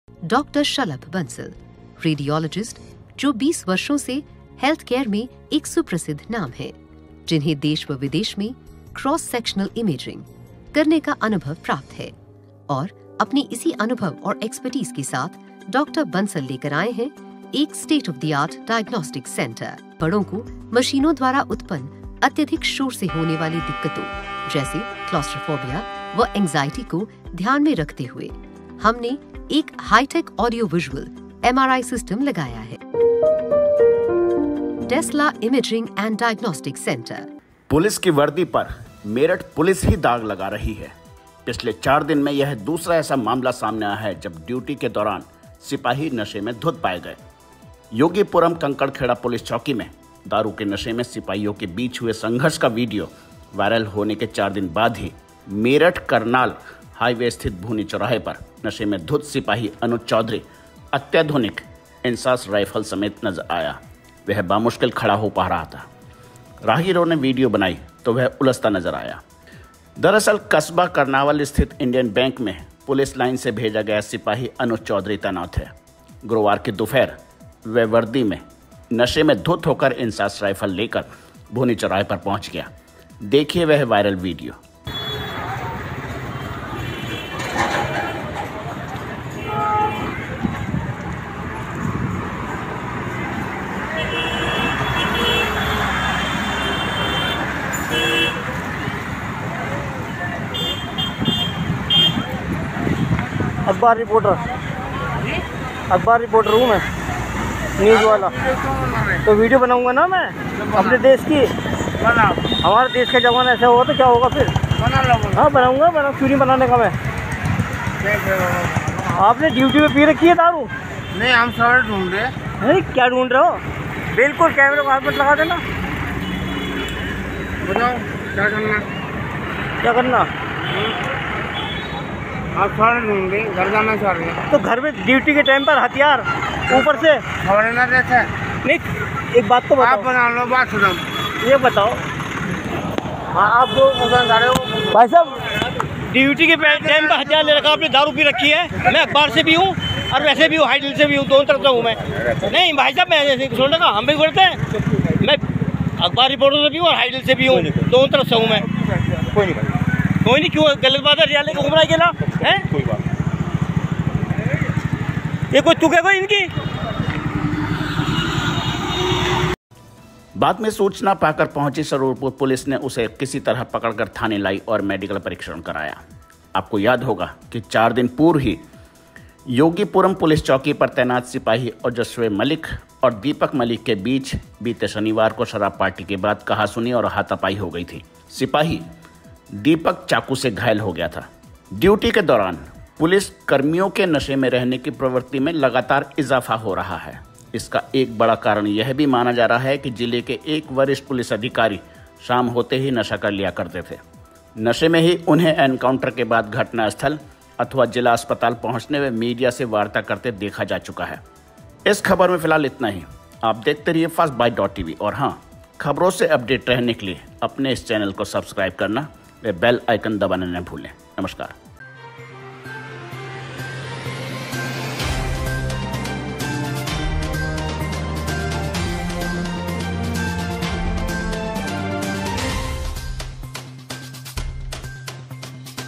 नशे में धुत्त सिपाही लहरा रहा था इंसास रायफल, राहगीरों ने वायरल किया वीडियो । Firstbyte.Tv
नशे में धुत्त सिपाही लहरा रहा था इंसास रायफल
करनावल के इंडियन बैंक में तैनात है सिपाही अनुज चौधरी
दिन में ही नशे में धुत्त मिला वर्दीधारी अनुज चौधरी
वायरल वीडियो में वह कैमरा छीनने की कोशिश करता आया नजर
राहगीरों ने वीडियो बनाकर की वायरल
नशे में सिपाहियों के रहने की प्रवृत्ति में हुआ इजाफा
चार दिन में यह दूसरी घटना आई सामने
योगीपुरम पुलिस चौकी पर नशे में सिपाहियों ने किया था एकदूसरे को घायल